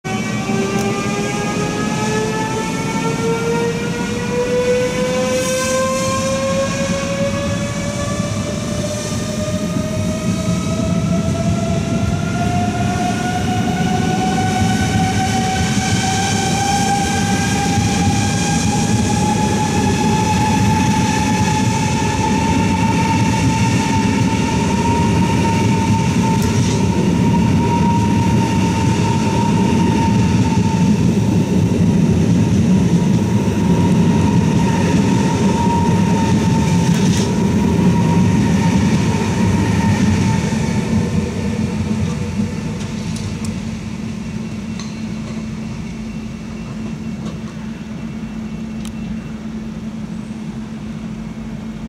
Eurostar 4013-4014 vertrekt uit Utrecht Centraal
Eurostar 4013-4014 vertrekt uit Utrecht Centraal richting Arnhem. ⇩⇩ Meer treinenvideo's ⇩⇩

⇩ Alle treinenseries ⇩